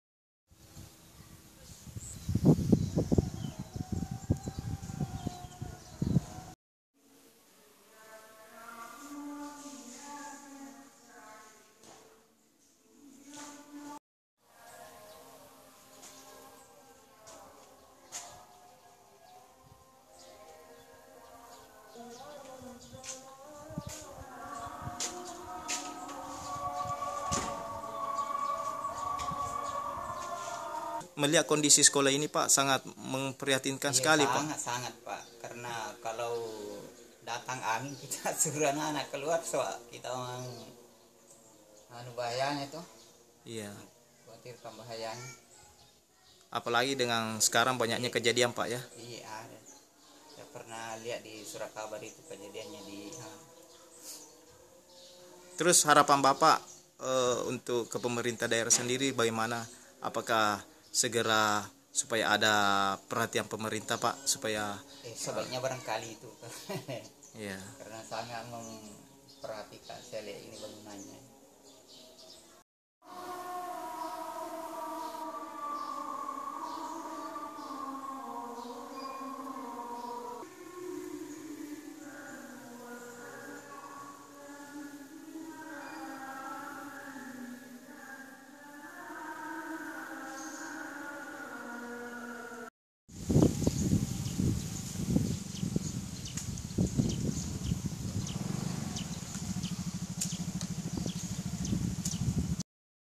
Kondisi Bangunan Sekolah di Bone Memprihatinkan
WATAMPONE.BONEPOS - Kondisi bangunan sekolah yang rapuh dan sudah tak layak digunakan sebagai sarana kegiatan belajar mengajar ternyata masih dapat ditemui di wilayah perkotaan di Kabupaten Bone, Sulawesi Selatan.

Setidaknya hal ini terjadi di Sekolah Dasar (SD) Inpres 3/77 Walannae. Para murid harus mengikuti kegiatan belajar dengan perasaan was-was, lantaran kondisi dinding bangunan sekolah yang retak sehingga sangat rawan ambruk.

Bahkan, jika angin kencang datang, murid-murid di Sekolah ini terpaksa diminta keluar dari kelas dan dipulangkan lebih awal. Pasalnya pihak sekolah khawatir tembok retak yang hampir terjadi diseluruh ruang kelas di sekolah tersebut ambruk dan menimpa para murid.

Seorang Guru di Sekolah tersebut, Darianti menuturkan, rapuhnya bangunan sekolah yang berada di Ibukota Kecamatan Tanete Riattang, Kabupaten Bone ini sudah terjadi sejak lebih dari setahun yang lalu.

Namun hingga kini pengajuan untuk rehabilitasi bangunan sekolah yang berdiri sejak tahun 1985 ini belum juga ditanggapi oleh Pemerintah setempat.

Pewarta Video : Suparman
Editor : Andi Dedhy